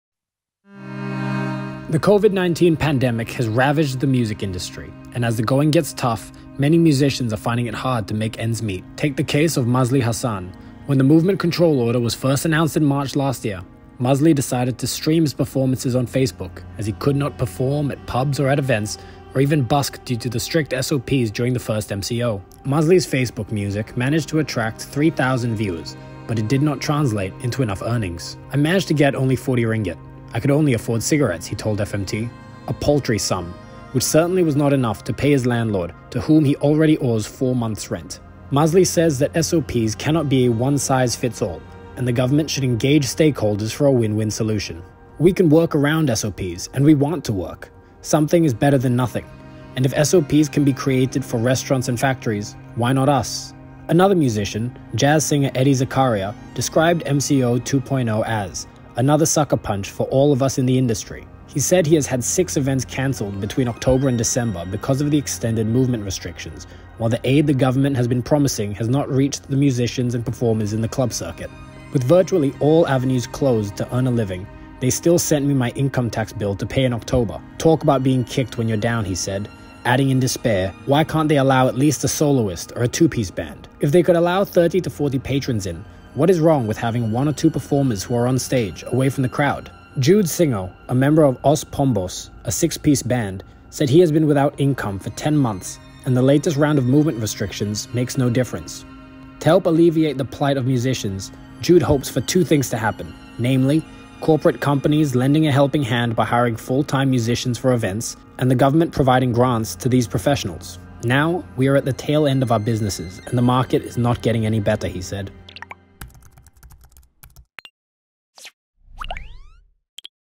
Musicians hit hard by Covid-19
The Covid-19 pandemic has ravaged the music industry with many musicians finding it hard to make ends meet.